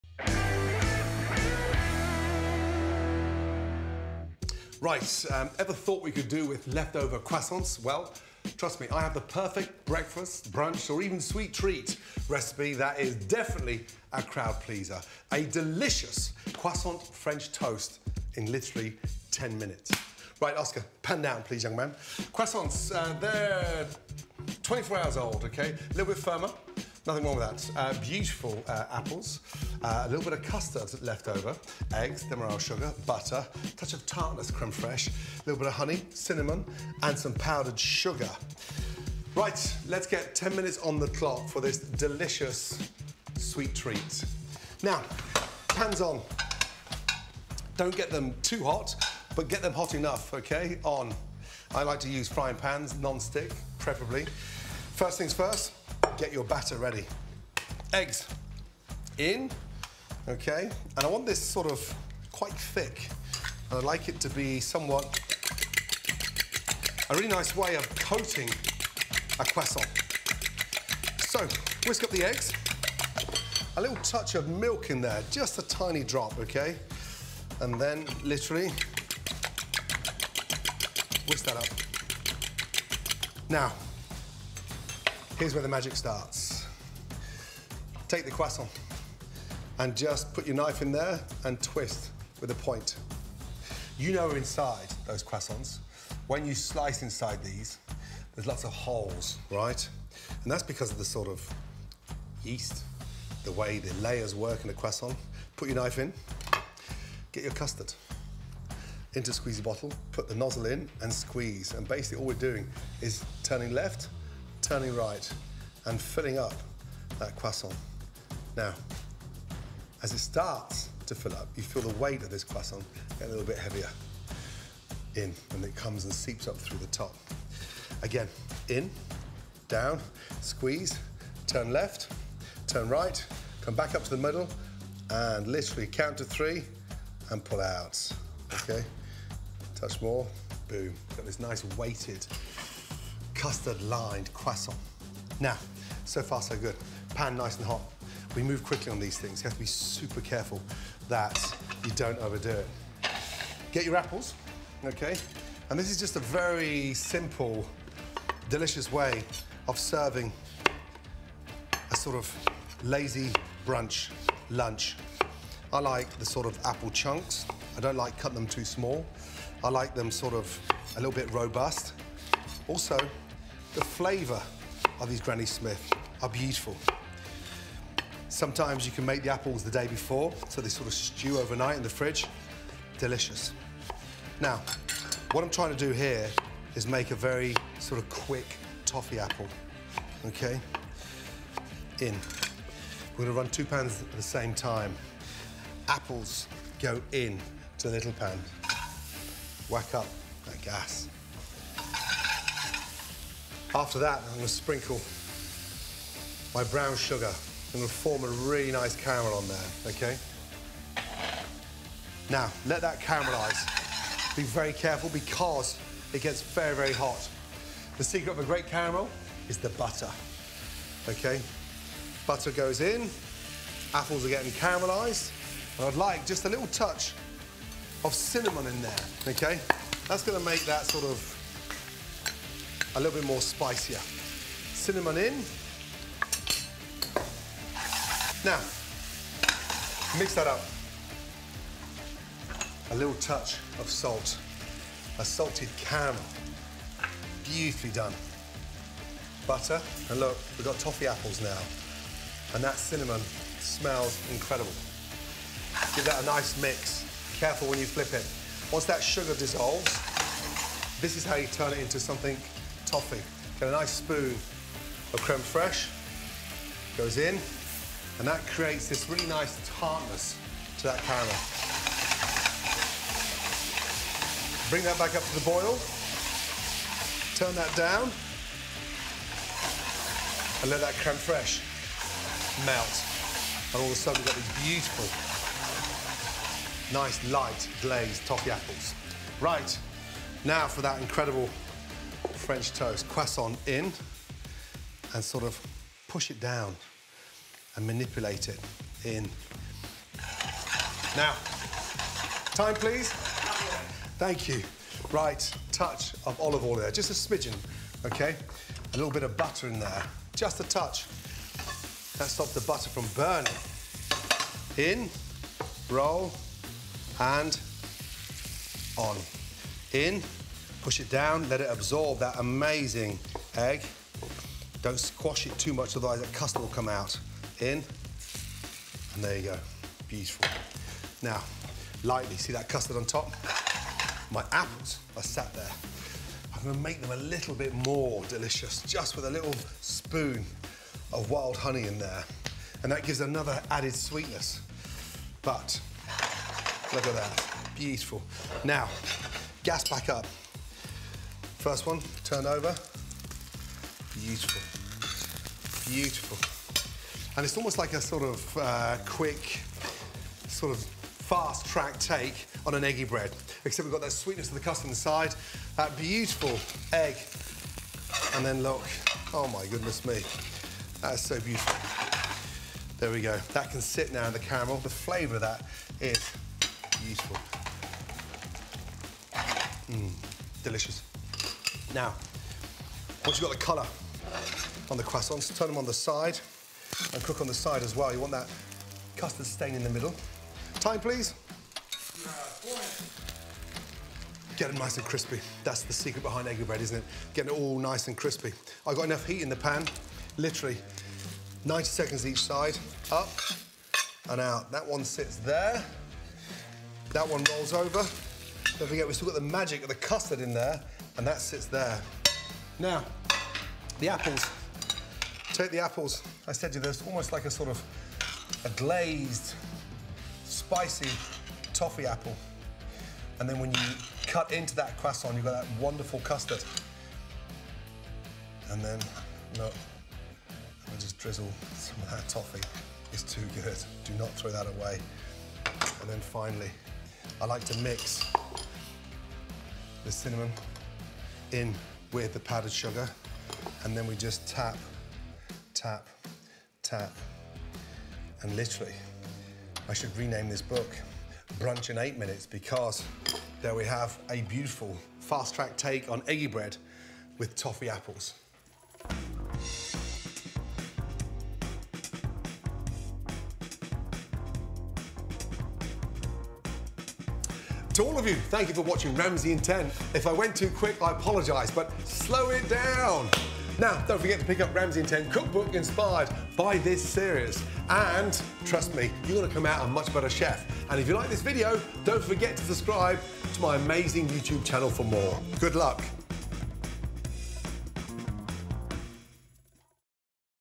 Stuffed Croissant French Toast Recipe in 7 Minutes ?!? | Gordon Ramsay
Brunch is served! Gordon is serving up a delicious Croissant French Toast in UNDER 10 minutes. This Ramsay in 10 dish is perfect for brunch, dessert or breakfast served stuffed and covered with caramelized apples. This is perfect for the weekends or with leftover croissants.